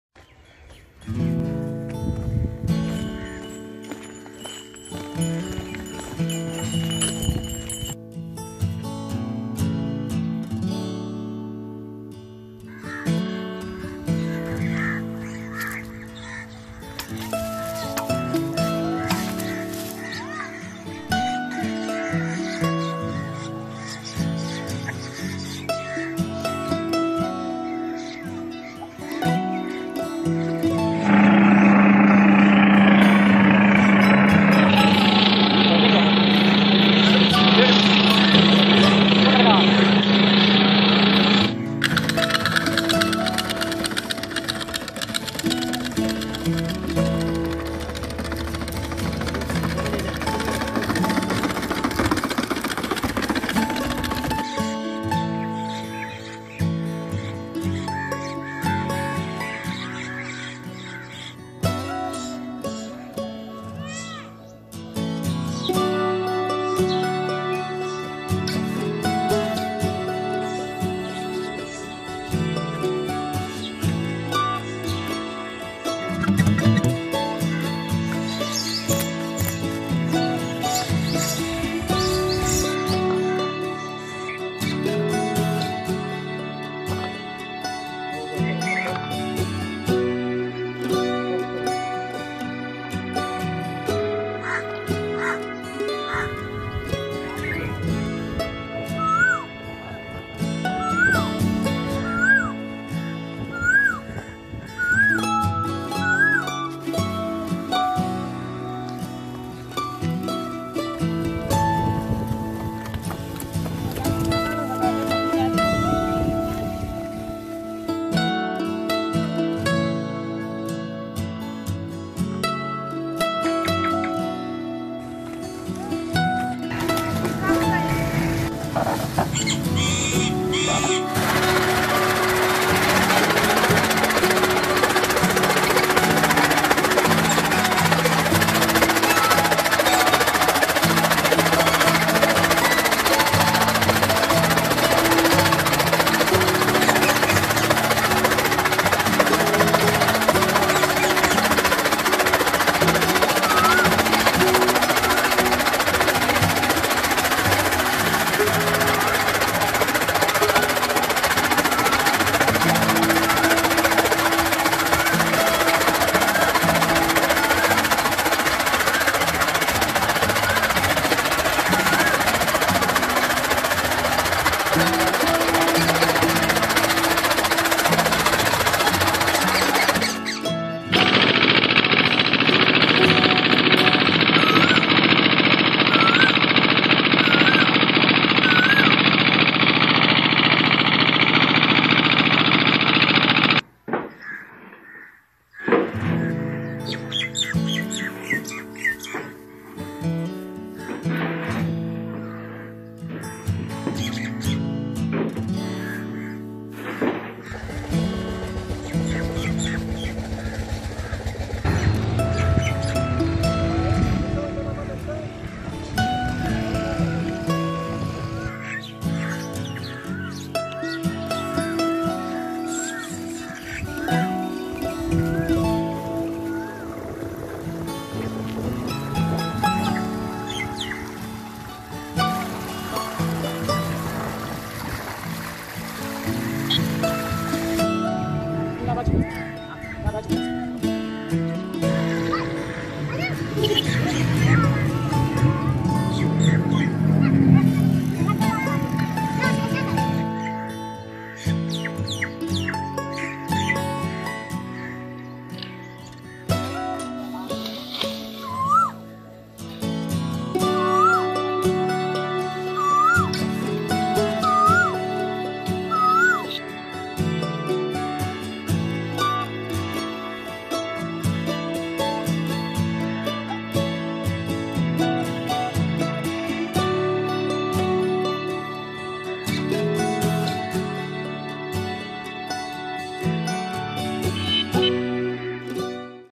Traditional Agriculture Tools And Amazing Farming Equipment | Old Farming Tools And Their Uses
Amazing ingenious agricalture tools/machinery and their uses.